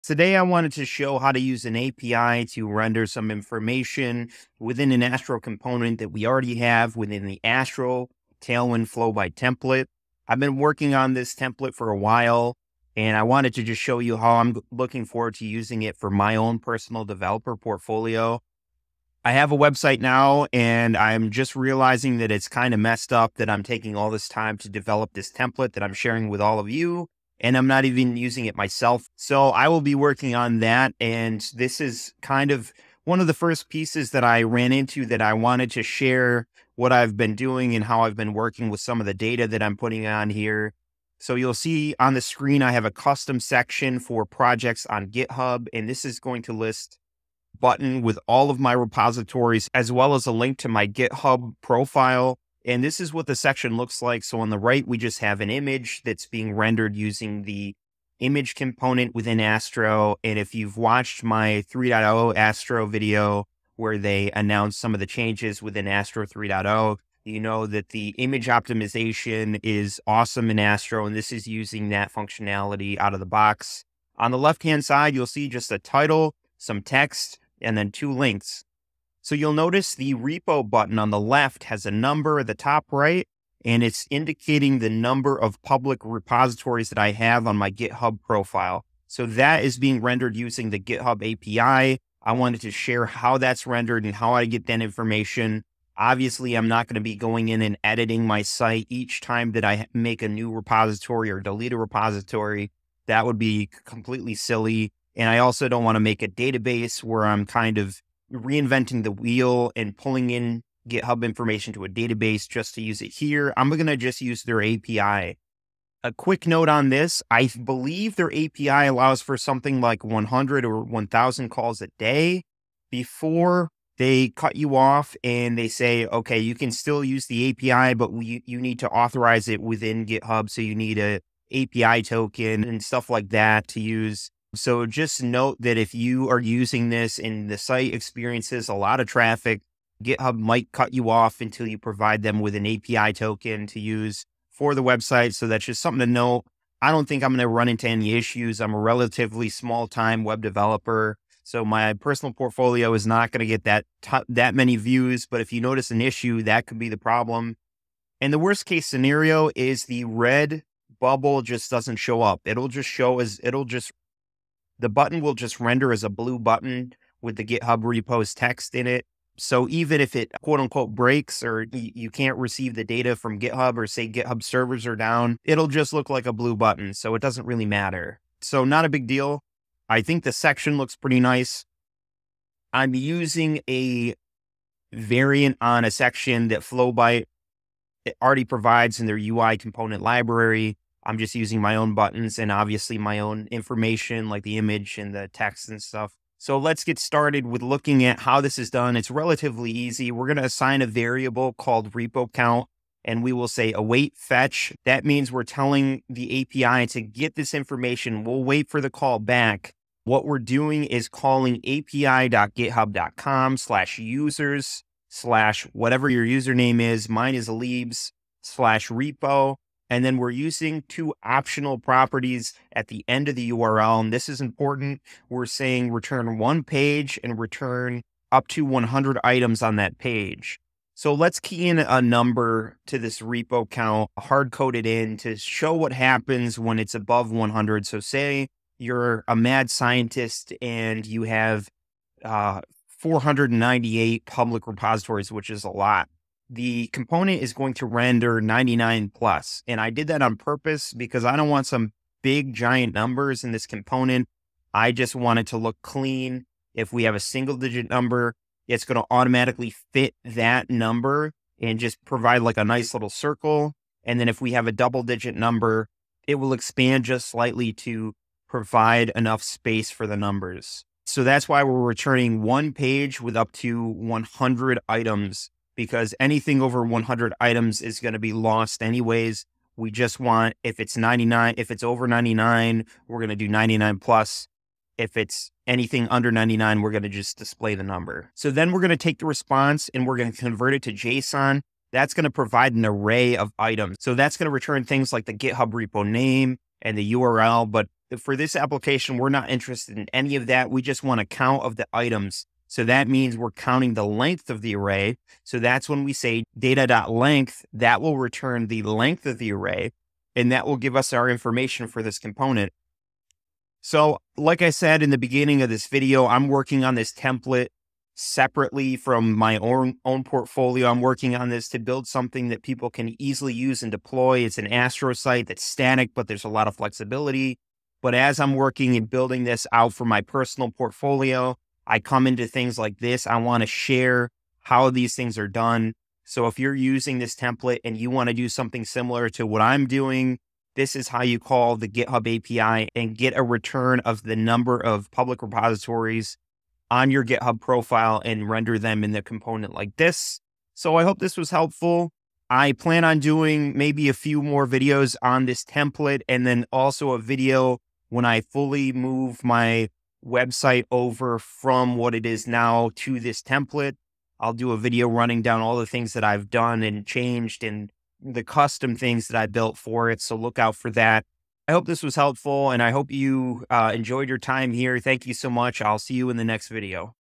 Fetching GitHub API data in Astro!
In this video we look at how to fetch some GitHub API data (user repos) and render a repo count into a component in the Astro-tailwind-flowbite template that I've been working on.

00:00 - Intro
00:41 - Overview
03:16 - API call
05:15 - Outro